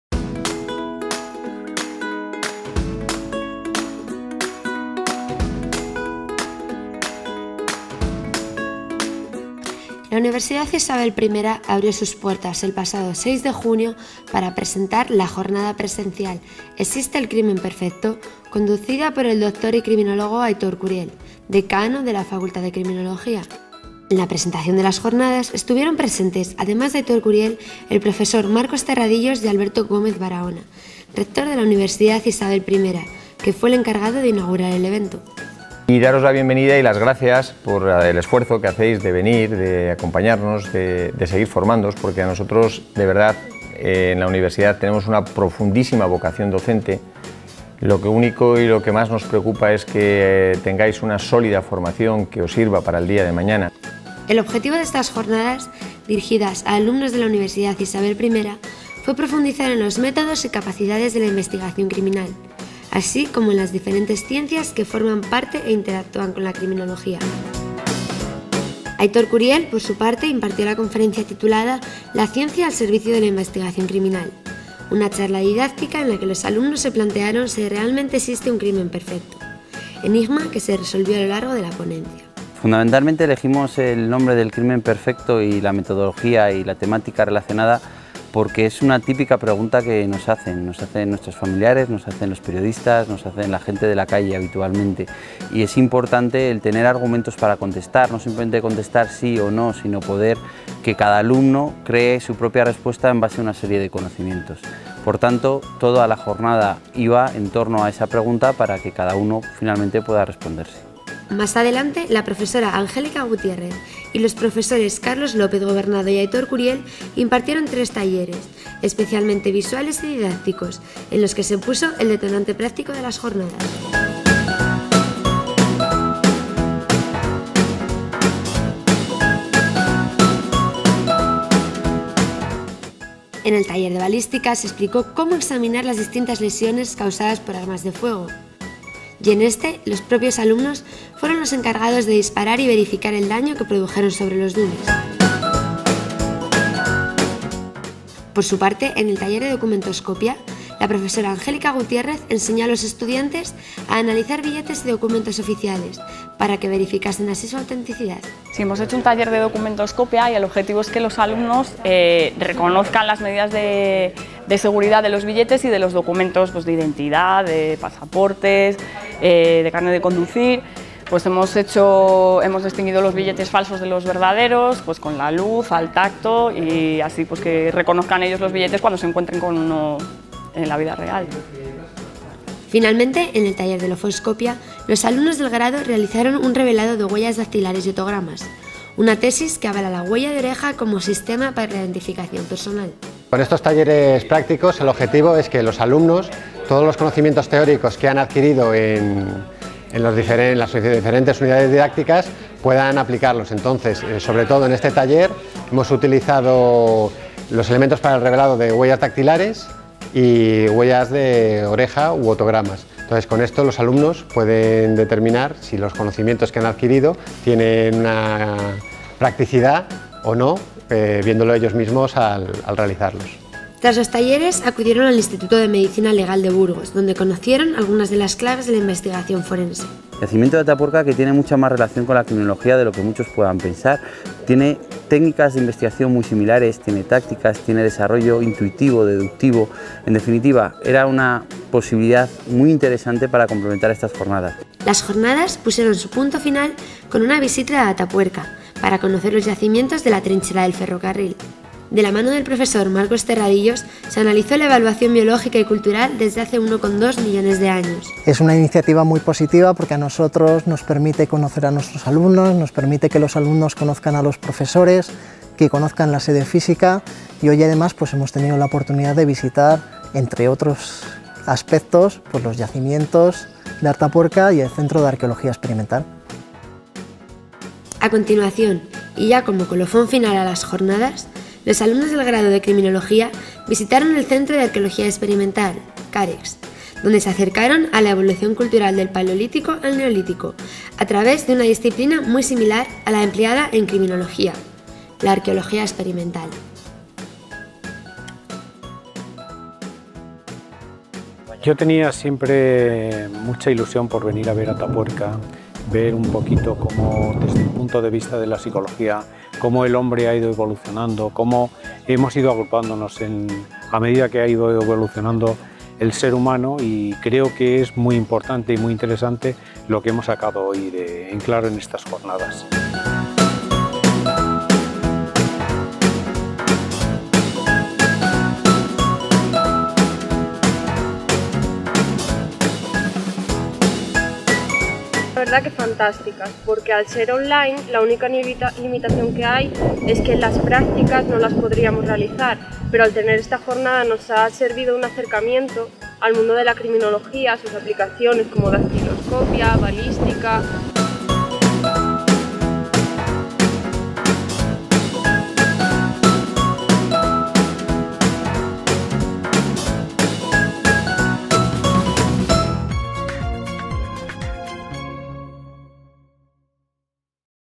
Jornada Presencial Criminología
La Universidad Isabel I abrió sus puertas el pasado 6 de junio para la Jornada Presencial ¿Existe el crimen perfecto? El objetivo de estas jornadas dirigidas a alumnos de la Universidad Isabel I, fue profundizar en los métodos y capacidades de la investigación criminal, así como las diferentes ciencias que forman parte e interactúan con la criminología.